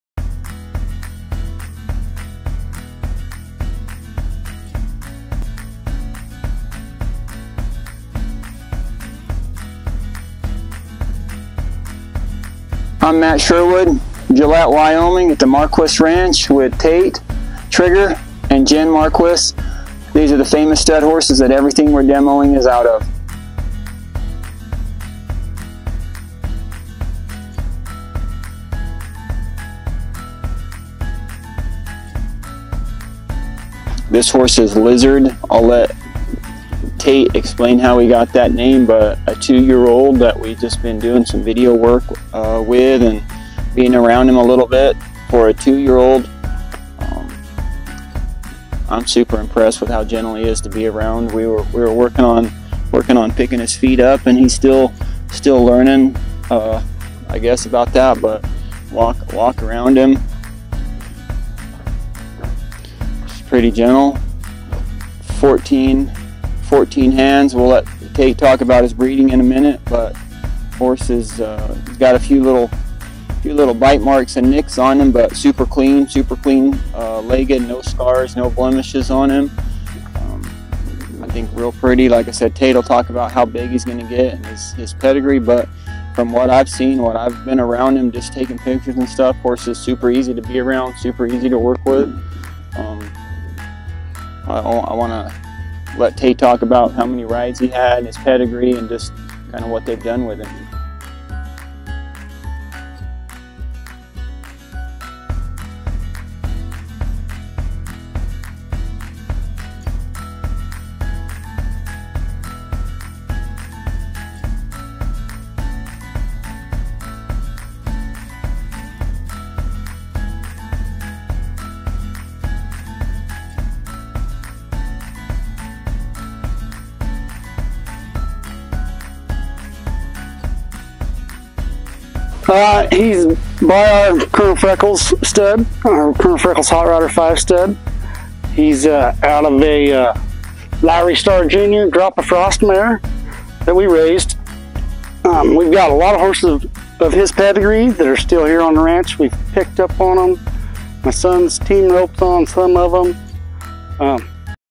Marquiss Quarter Horses - Lot #4 A Bunkhouse Story - Gold Buckle Pro Demo
Lizard has been started lightly under saddle and has a calm, willing attitude. We have had the pleasure of raising and using a lot of horses in his pedigree, and they have all been such a pleasure to ride and handle. They all have the same gentle, tough, willing, and trainable attitude that Lizard is showing us now. We have used them as our go to horses for years. Lizards sire, Hotrod Freckle, is one of the gentlest,levelheaded stallions we have ever had the pleasure of owning. He is a money earning rope horse that excels on the heel side. From ranch horses, money earning rope horses, pick-up horses, to polo ponies, they have excelled and never refused anything we have asked of them. Lizard is well on is way to following in their footsteps!